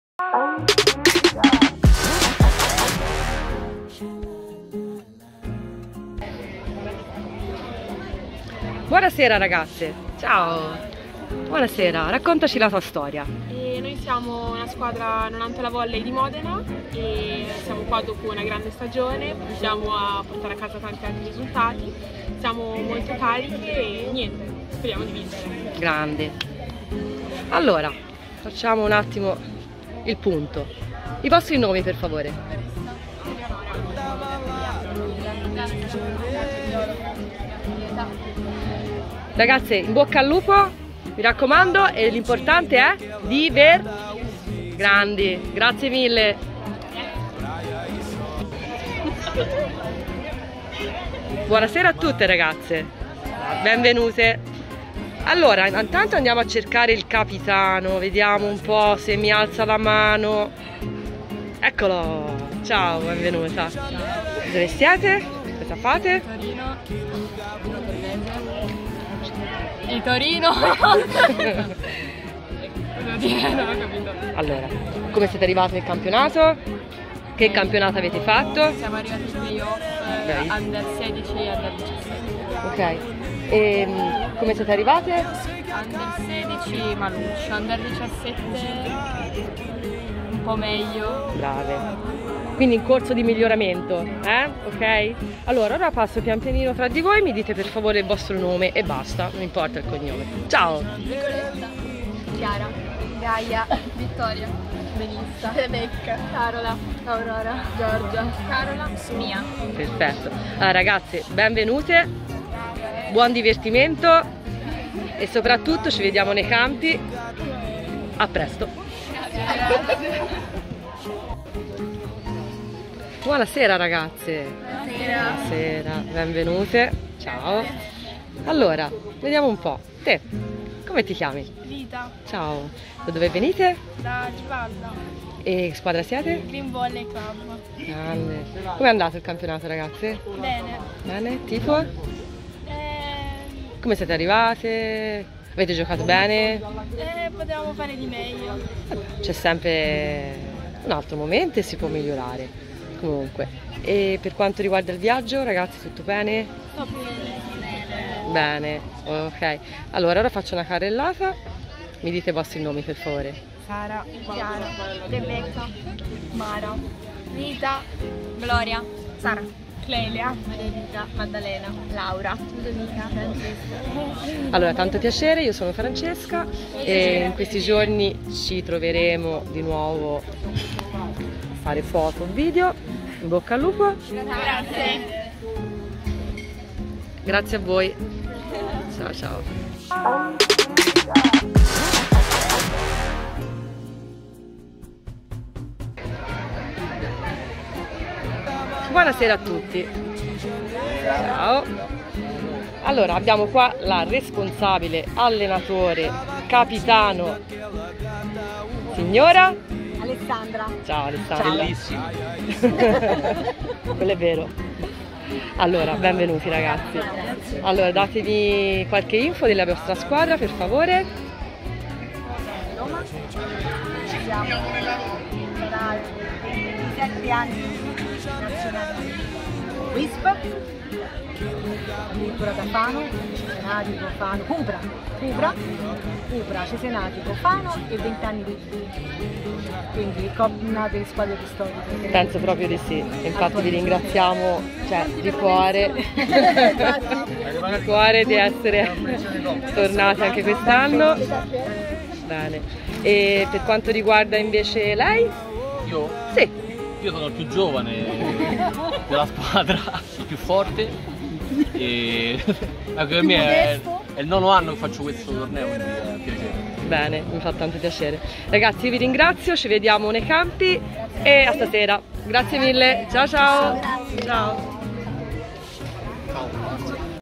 Sportpertutti Fest 2024:Chi sono le squadre che partecipano ai Campionati nazionali della Pallavolo?
Interviste a cura di Francesca Galvagno